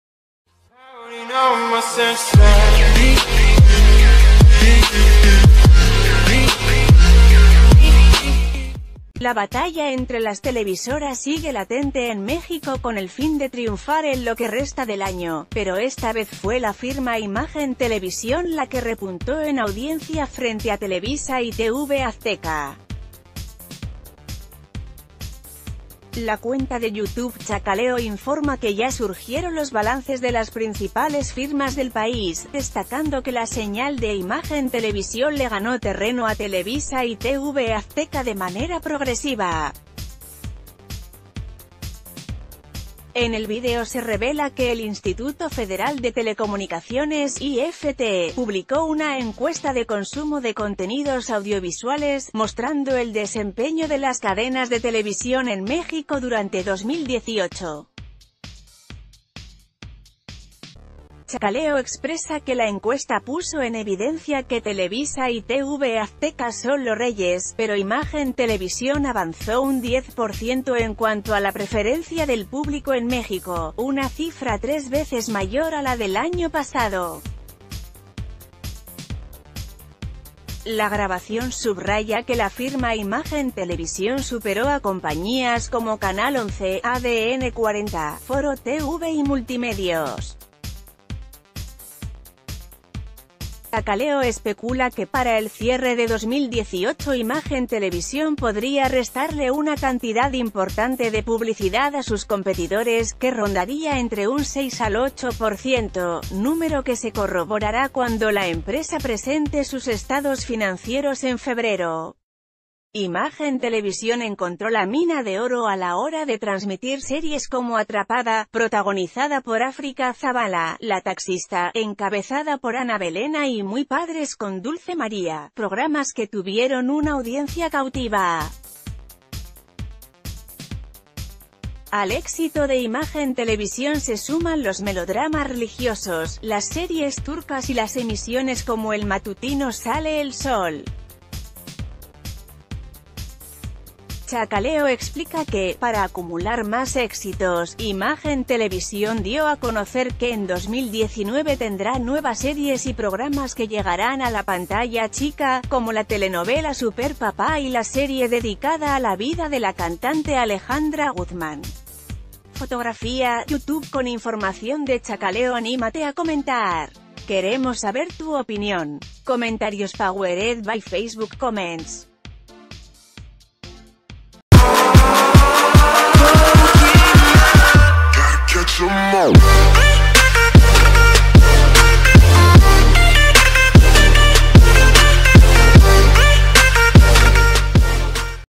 Imagen Televisión da duro golpe a audiencia de Televisa y TV Azteca en 2018
La batalla entre las televisoras sigue latente en México con el fin de triunfar en lo que resta del año, pero esta vez fue la firma Imagen Televisión la que repuntó en audiencia frente a Televisa y TV Azteca. La cuenta de YouTube Chacaleo informa que ya surgieron los balances de las principales firm...